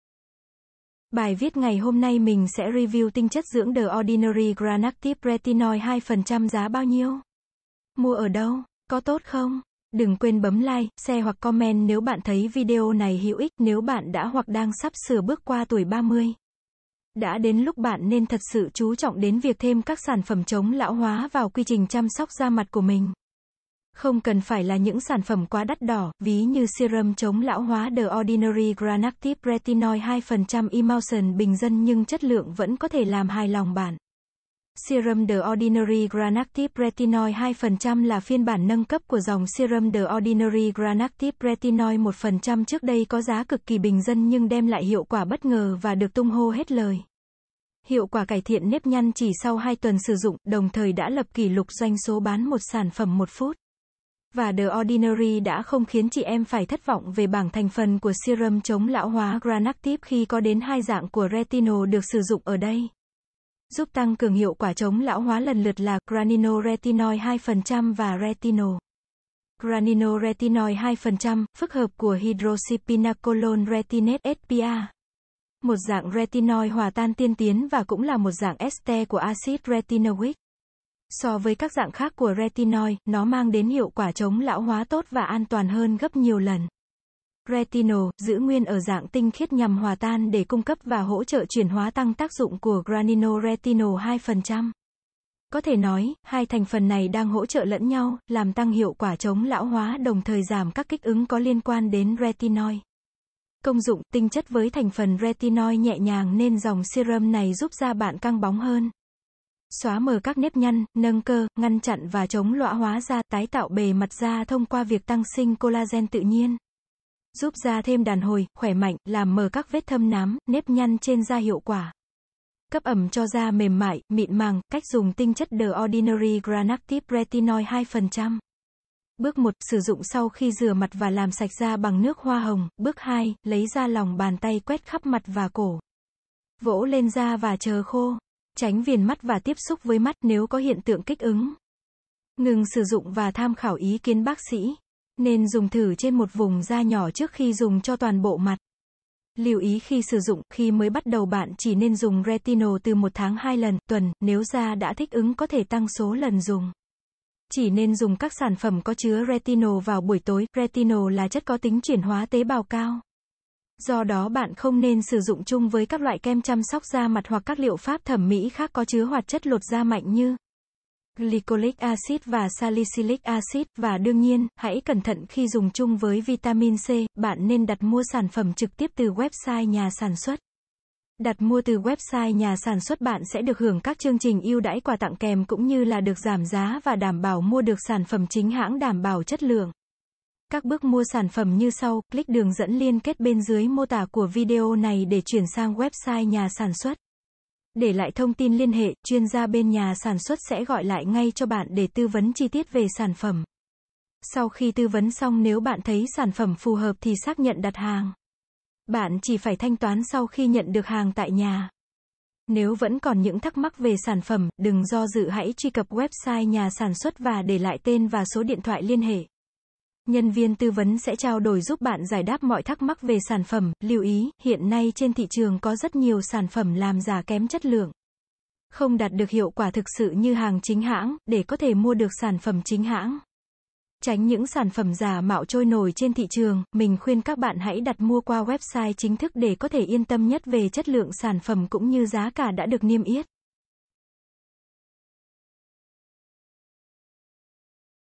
Review Tinh Chất Dưỡng The Ordinary Granactive Retinoid 2% Giá bao nhiêu? Mua ở đâu? Có tốt không?
Bài viết ngày hôm nay mình sẽ Review Tinh Chất Dưỡng The Ordinary Granactive Retinoid 2% Giá bao nhiêu? Mua ở đâu? Có tốt không?
    Nếu bạn đã hoặc đang sắp sửa bước qua tuổi 30,  đã đến lúc bạn nên thật sự chú trọng đến việc thêm các sản phẩm chống lão hóa vào quy trình chăm sóc da mặt của mình. Không cần phải là những sản phẩm quá đắt đỏ, ví như serum chống lão hóa The Ordinary Granactive Retinoid 2% Emulsion bình dân nhưng chất lượng vẫn có thể làm hài lòng bạn.

     Serum The Ordinary Granactive Retinoid 2% là phiên bản nâng cấp của dòng Serum The Ordinary Granactive Retinoid 1% trước đây có giá cực kỳ bình dân nhưng đem lại hiệu quả bất ngờ và được tung hô hết lời. Hiệu quả cải thiện nếp nhăn chỉ sau 2 TUẦN sử dụng, đồng thời đã lập kỷ lục doanh số bán 1 sản phẩm/phút.
 Và The Ordinary đã không khiến chị em phải thất vọng về bảng thành phần của serum chống lão hoá Granactive khi có đến hai dạng của retinol được sử dụng ở đây, giúp tăng cường hiệu quả chống lão hoá lần lượt là: Granino Retinoid 2% và Retinol.

·   Granino Retinoid 2%: phức hợp của Hydroxypinacolone Retinoate (HPR) – một dạng retinoid hòa tan tiên tiến và cũng là một dạng este của axit retinoic. So với các dạng khác của retinoid, nó mang đến hiệu quả chống lão hóa tốt và an toàn hơn gấp nhiều lần.

·   Retinol: giữ nguyên ở dạng tinh khiết nhằm hoà tan để cung cấp và hỗ trợ chuyển hoá tăng tác dụng của Granino Retinol 2%.

   Có thể nói, hai thành phần này đang hỗ trợ lẫn nhau, làm tăng hiệu quả chống lão hoá đồng thời giảm các kích ứng có liên quan đến retinoid.
CÔNG DỤNG

-  TINH CHẤT với thành phần Retinoid nhẹ nhàng nên dòng serum này giúp da bạn căng bóng hơn, XÓA MỜ CÁC NẾP NHĂN, NÂNG CƠ, ngăn chặn và chống lõa hóa da.
-  Tái tạo bề mặt da thông qua việc tăng sinh Collagen tự nhiên, giúp da thêm đàn hồi, khỏe mạnh.
-  Làm mờ các vết THÂM NÁM, nếp nhăn trên da hiệu quả.
-  Cấp ẩm cho da mềm mại, mịn màng.
Cách dùng tinh chất The Ordinary Granactive Retinoid 2%
Bước 1: Sử dụng sau khi rửa mặt và làm sạch da bằng nước hoa hồng.

Bước 2: Lấy ra lòng bàn tay quét khắp mặt và cổ, vỗ lên da và chờ khô. Tránh viền mắt và tiếp xúc với mắt

*** Nếu có hiện tượng kích ứng, ngưng sử dụng và tham khảo ý kiến bác sĩ. Nên dùng thử trên một vùng da nhỏ trước khi dùng cho toàn bộ mặt.

Lưu ý khi sử dụng:
-  Khi mới bắt đầu bạn chỉ nên dùng Retinol từ 1- 2 lần/ tuần, nếu da đã thích ứng có thể tăng số lần dùng. Chỉ nên dùng các sản phẩm có chứa Retinol vào buổi tối.

-  Retinol là chất có tính chuyển hóa tế bào cao, do đó bạn không nên sử dụng chung với các loại kem chăm sóc da mặt hoặc các liệu pháp thẩm mỹ khác có chứa hoạt chất lột da mạnh như : Glycolic Acid và Salicylic Acid, và đương nhiên, hãy cẩn thận khi dùng chung với vitamin C.